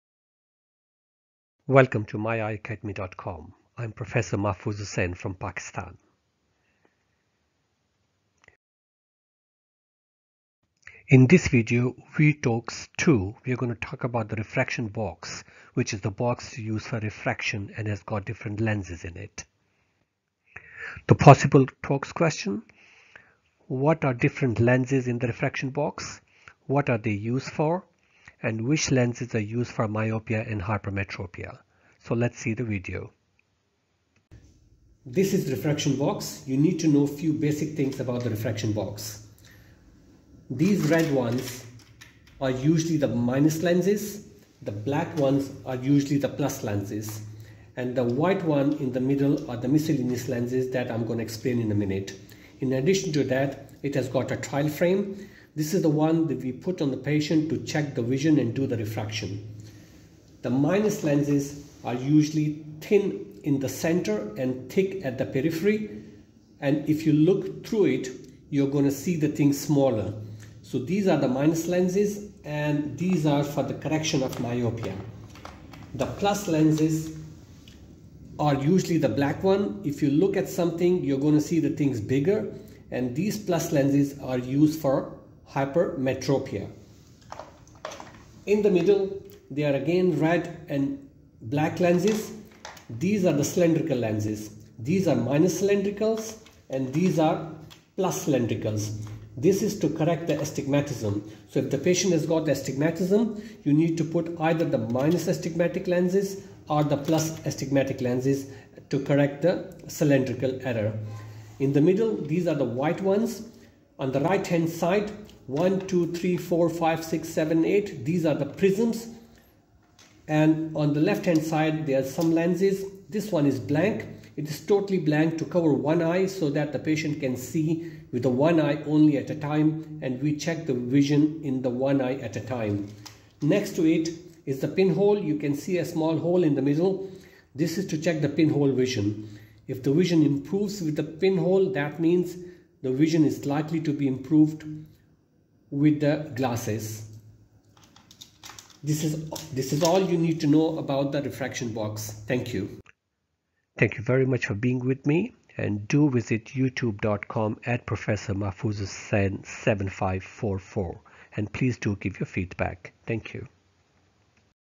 TOACS 002
Basic details of refraction set. Different lenses in refraction box and their uses in refraction